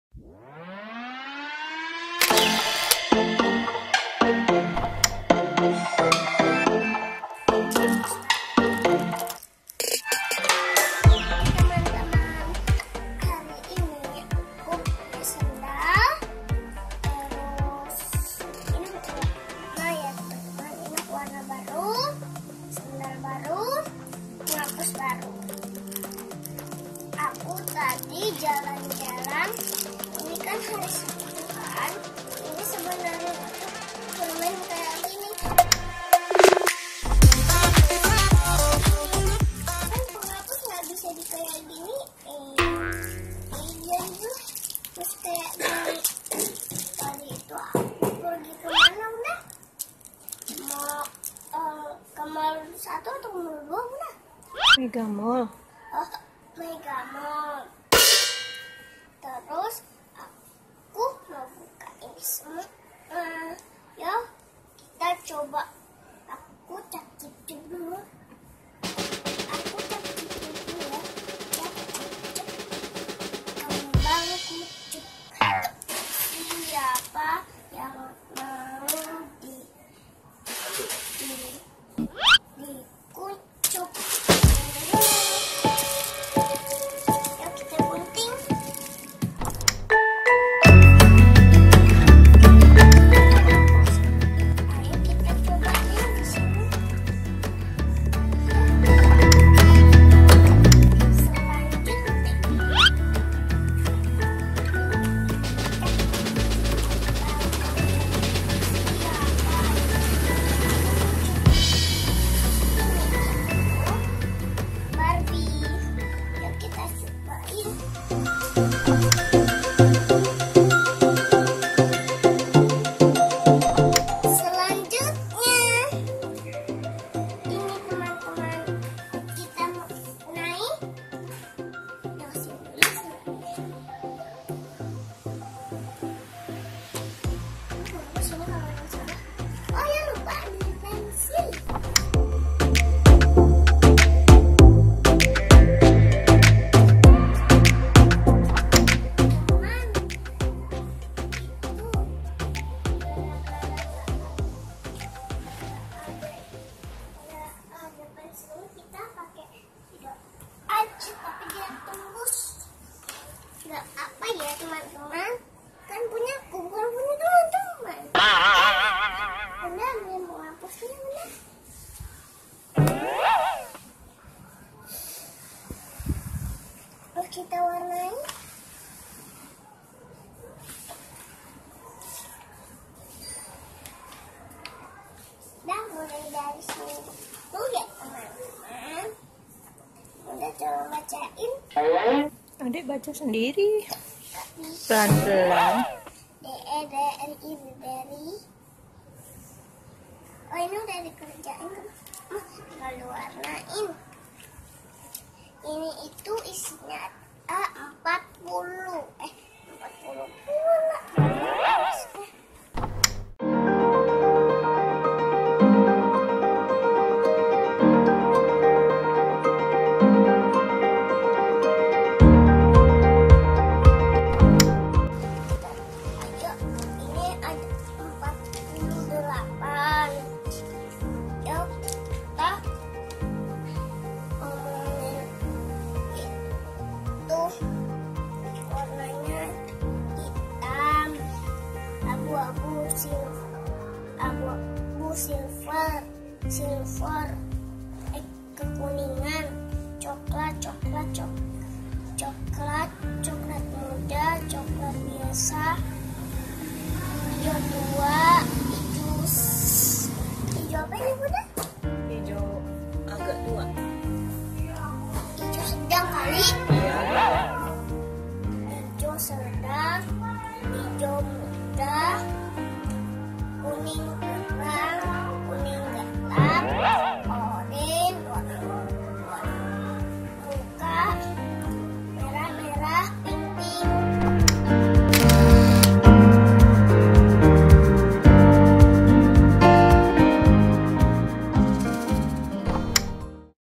CAP CIP CUP! PILIH PENSIL WARNA, SANDAL BARU ATAU PENGHAPUS MODEL ULAR??
YURA baru beli tiga barang di Megamall Batam. Ada pensil warna, sandal dan penghapus baru model ular.

Bingung mau cobain duluan yang mana??